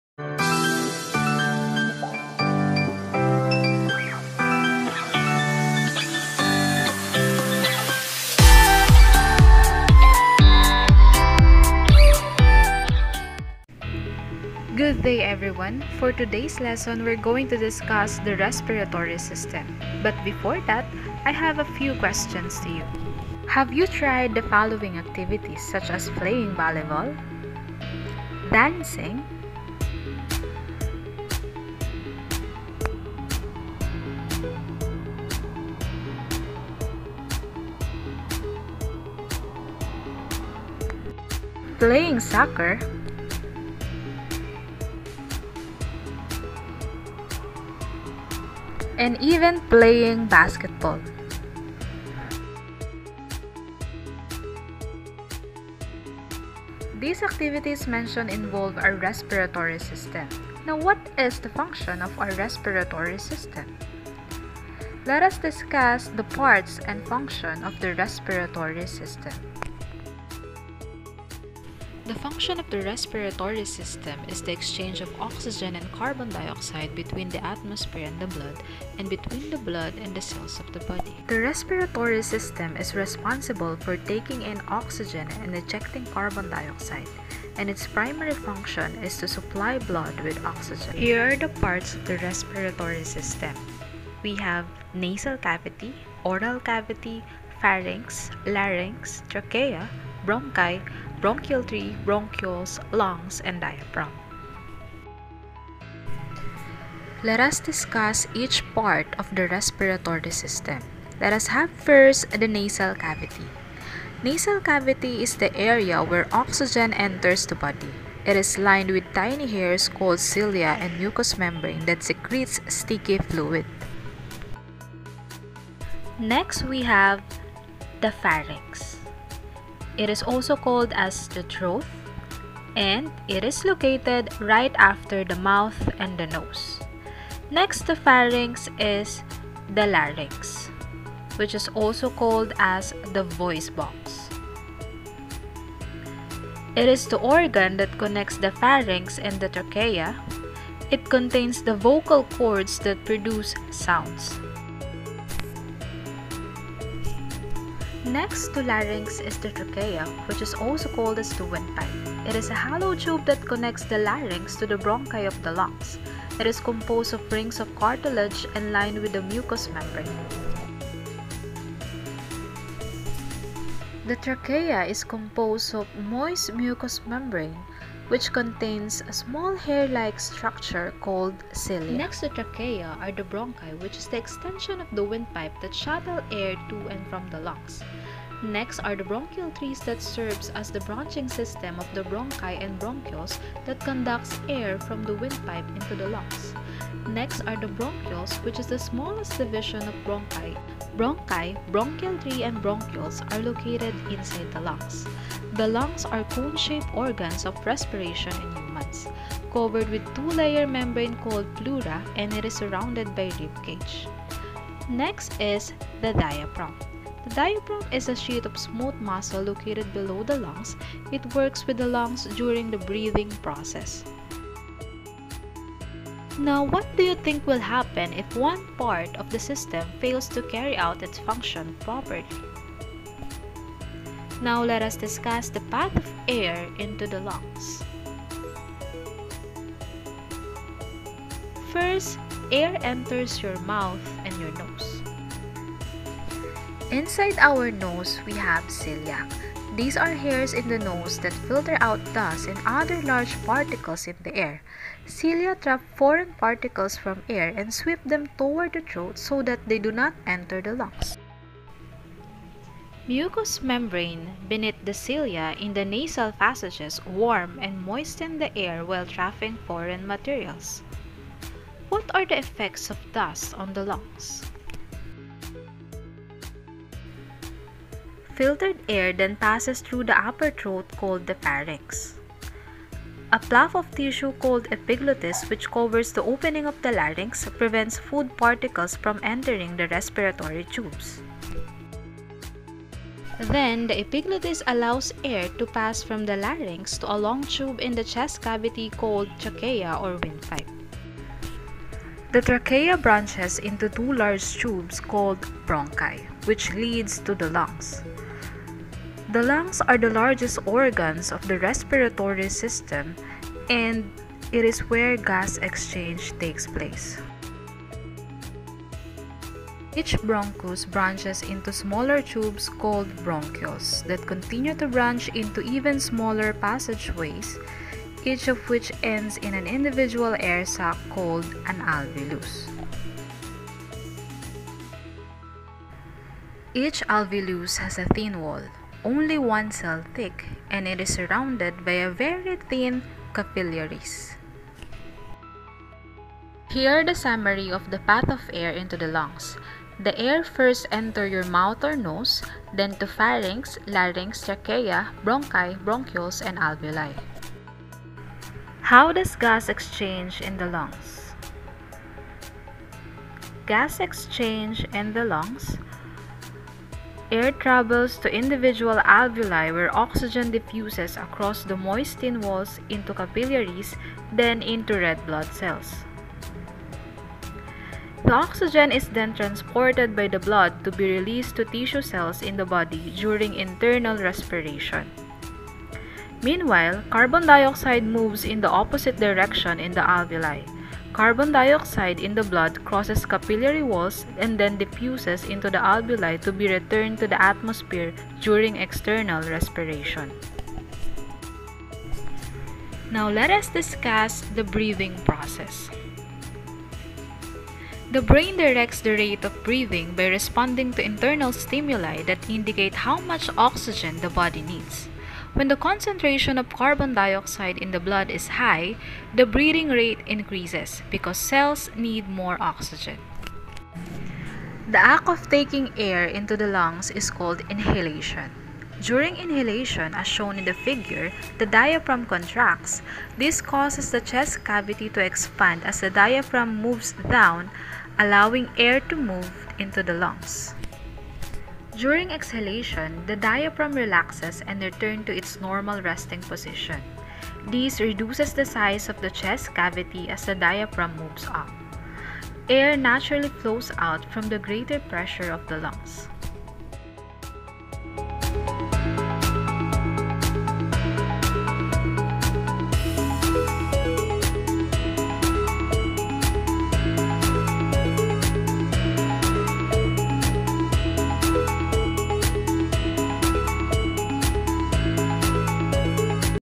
Parts and Functions of the respiratory system
The respiratory system is made up of nasal passages, nasal passages, warm and moisten the air while trapping foreign materials. Pharynx, larynx, epiglottis, trachea, lungs, bronchi, bronchioles, alveoli, and diaphragm. Air travels from the outside environment to the lungs where it passes through the alveoli as shown in the figure above.
First, air enters your mouth or nose. There are hairs in the nose that filter out
dust and other large particles in the air. These hair-like structures are called
cilia, and also line in the nasal passages. Cilia trap foreign particles from air and sweep them toward the throat so that they do not enter the lungs. Mucous membranes beneath the cilia in the nasal passages warm and moisten the air while trapping foreign materials.

Filtered air then passes through the upper throat called the pharynx. A flap
of tissue called epiglottis, which covers the opening of the larynx, prevents food particles from entering the respiratory tubes. The epiglottis allows air to pass from the larynx to a long tube in the chest cavity called the trachea, or windpipe. The trachea branches into two large tubes called bronchi (singular, bronchus) which lead to the lungs. The lungs are the largest organs of the respiratory system, and gas exchange takes place in the lungs. Each bronchus branches into smaller tubes called bronchioles.

Each of these small tubes continues to branch into even smaller passageways, each of which ends in an individual air sac, called an alveolus (plural, alveoli).Each alveolus has a thin wall- only one cell thick, and is surrounded by very thin capillaries